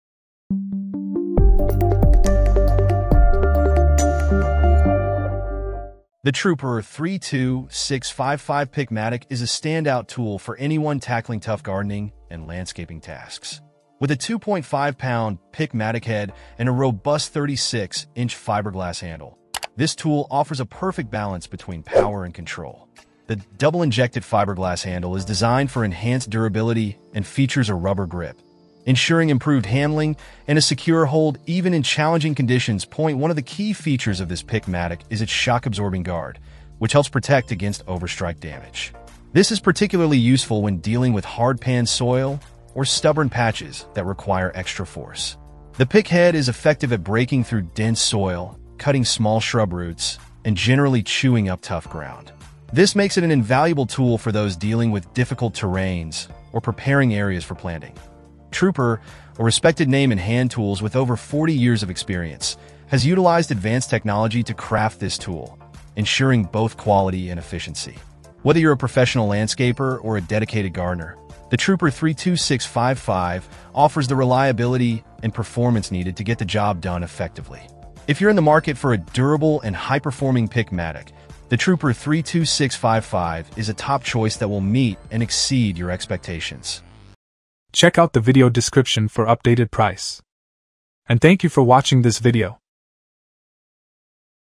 Truper 32655 2-1/2-Pound Pick Mattock: The Best Tool for Stubborn Soil
0:00 Intro
0:06 Review

Things that we mentioned in this video:
Truper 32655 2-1/2-Pound Pick : B005Q7CM8K